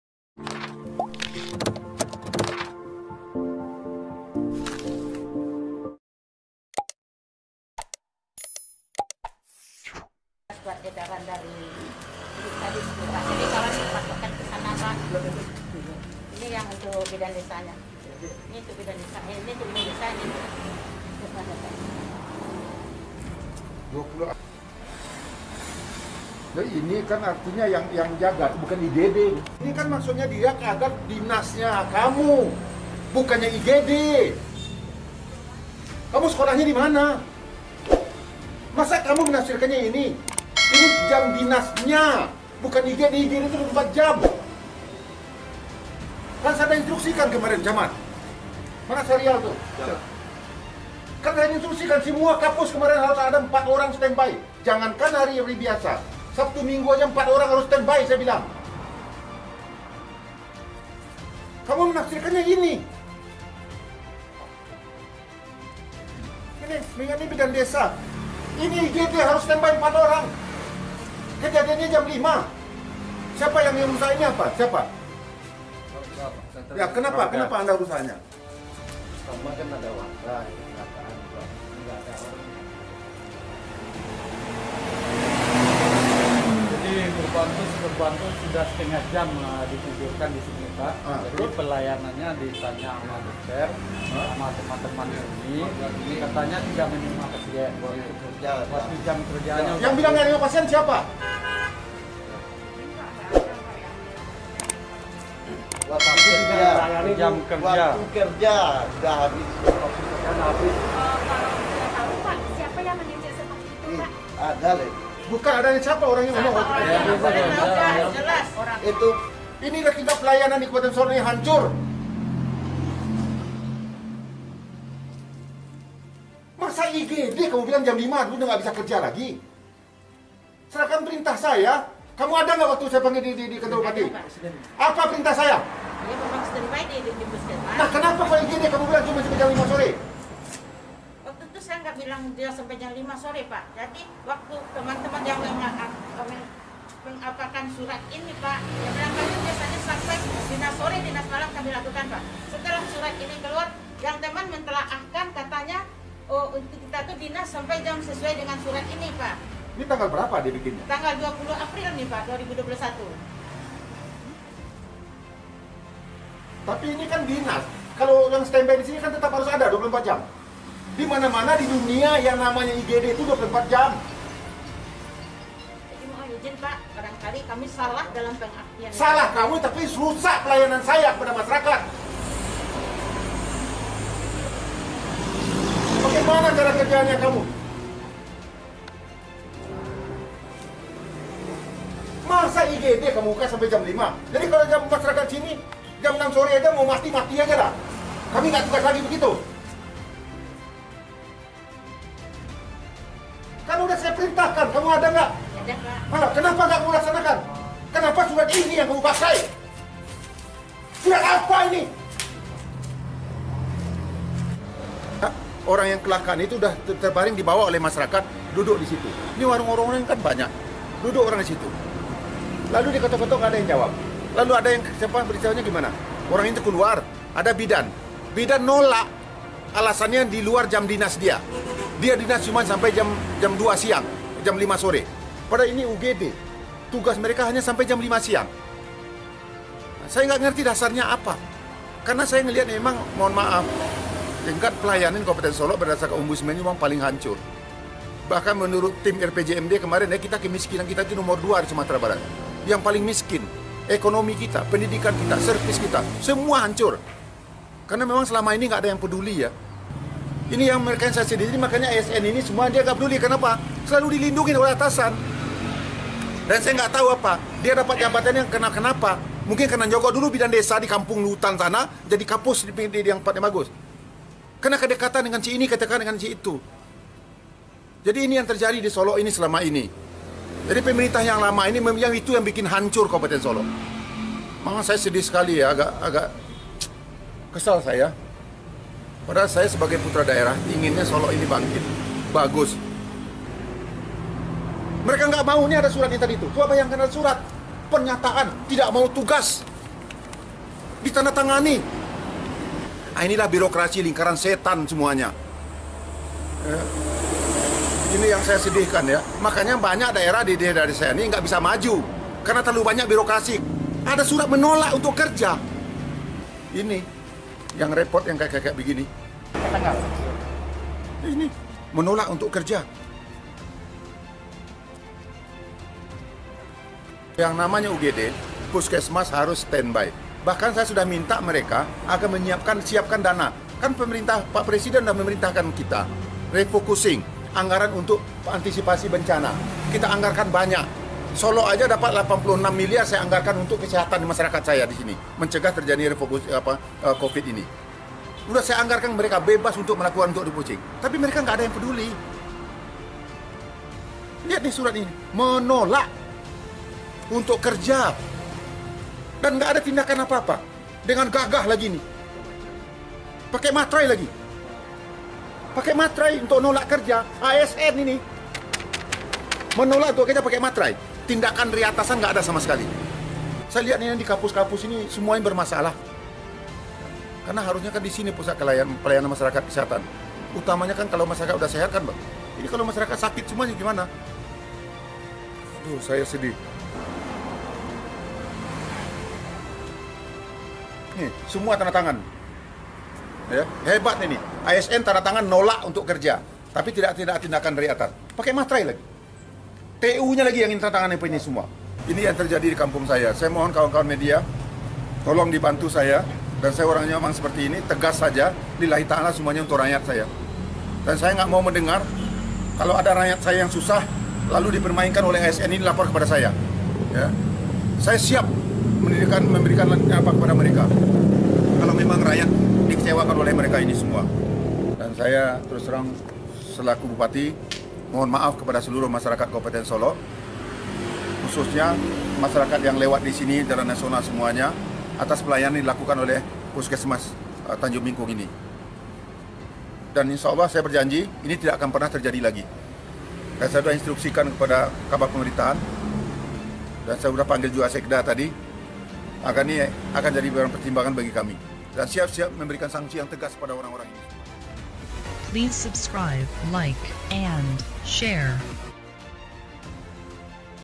Bupati Solok Meradang❗Petugas Puskesmas Tak Buka UGD
Petugas Puskesmas Tanjung Bingkung, Kecamatan Kubung, Kabupaten Solok, Sumatera Barat tidak mau bekerja di Unit Gawat Darurat (UGD) atau bekerja hanya sampai pukul 14.00 WIB. Padahal UGD harus buka 24 jam.

Mirisnya, kecelakaan lalu lintas terjadi tepat di depan Puskesmas. Korban yang mengalami luka dibawa warga ke UGD Puskesmas tersebut. Namun, petugas malah mengatakan, tidak bisa melayani karena sudah di luar jam kerja.

Bahkan korban dibiarkan tegeletak di luar ruangan UDG. Melihat hal itu, warga setempat mengamuk, dan memecahkan kaca Puskesmas. Korban terpaksa dibawa ke Puskesmas lain. Mendapat laporan tersebut, Bupati Solok Epyardi Asda melakukan sidak ke Puskesmas.

Yang membuat Epy terkejut, ada surat kesepakatan bersama petugas kesehatan di Puskesmas itu tidak mau bekerja di UGD dengan alasan tidak ada pasien di UGD. Mendapati kurangnya pelayanan itu, Bupati Solok mengamuk dan akan memberikan sangsi tegas kepada pihak Puskesmas.